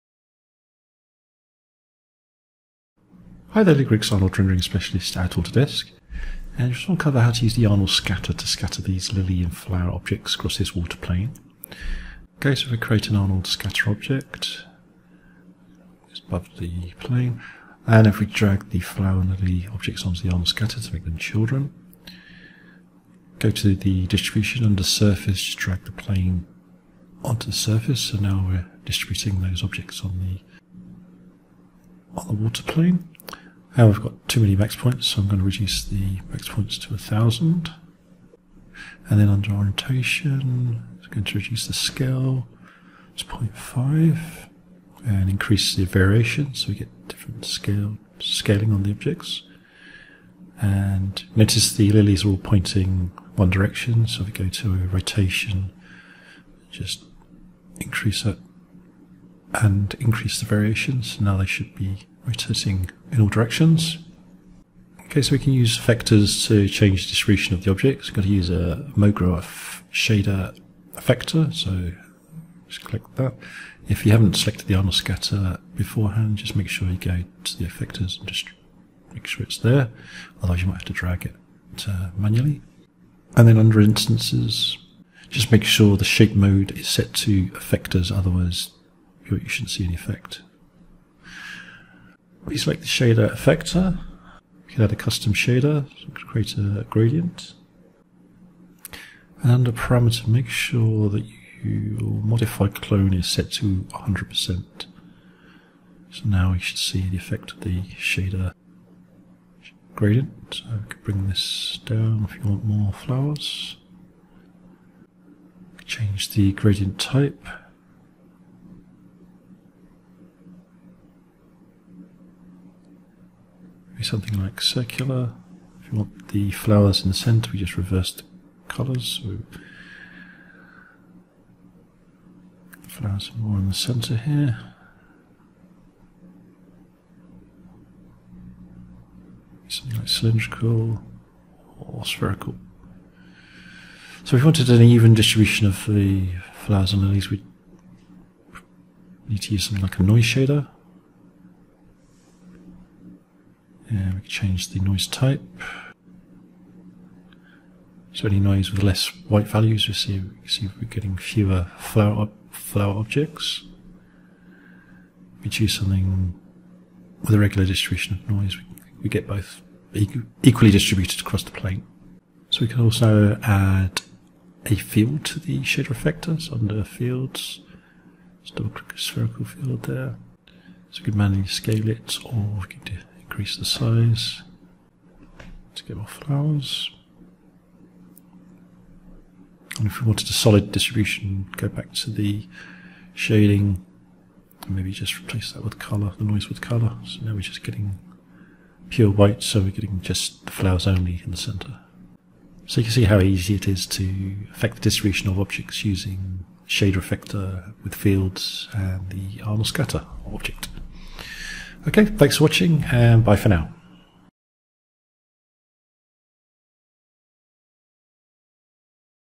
Arnold Tutorial - Shader Effector with Arnold Scatter in Cinema 4D (GPU)
This tutorial shows how to use the Arnold Scatter with the Mograph Shader Effector to distribute some lily objects across a pond of water.